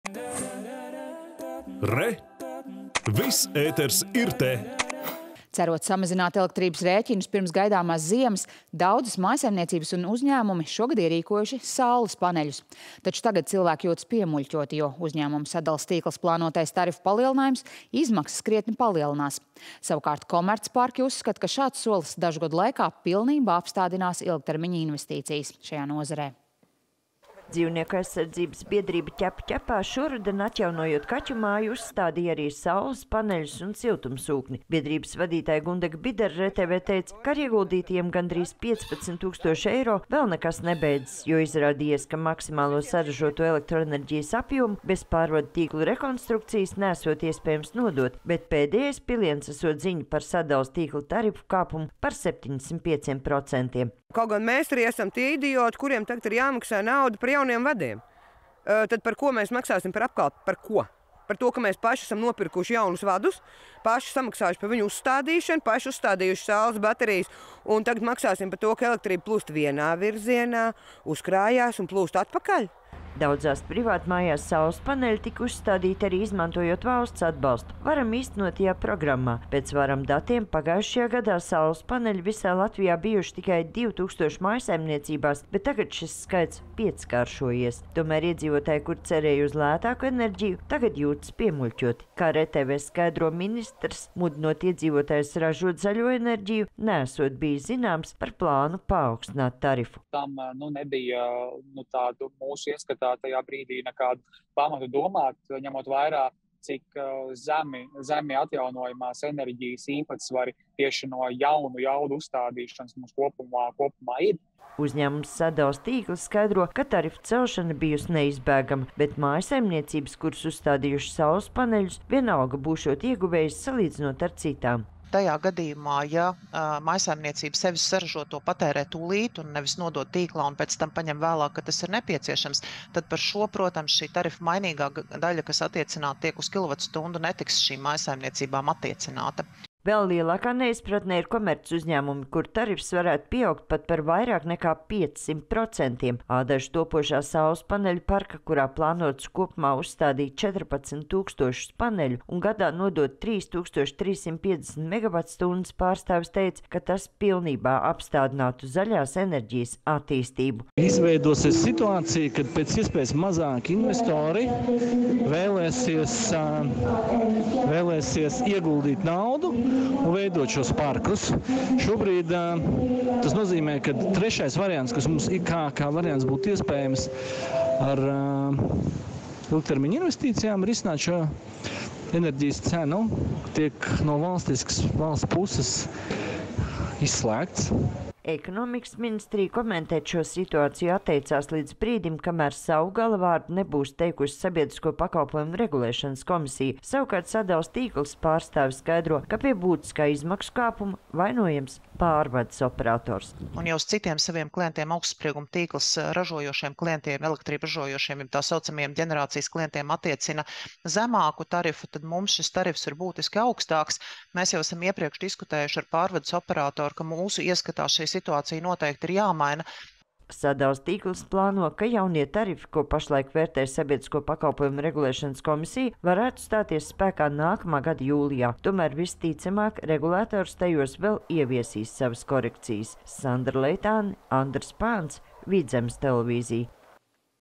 Vai saules paneļu ierīkošana būs izdevīga arī turpmāk?
Cerot uz lētāku elektroenerģiju, daudzas mājsaimniecības un uzņēmumi ierīkojuši saules paneļus. Taču tagad cilvēki jūtas piemuļķoti, jo uzņēmuma “Sadales tīkls” plānotais tarifu palielinājums izmaksas krietni palielinās. Savukārt komercparki uzskata, ka šāds solis dažu gadu laikā pilnībā apstādinās ilgtermiņa investīcijas šajā nozarē.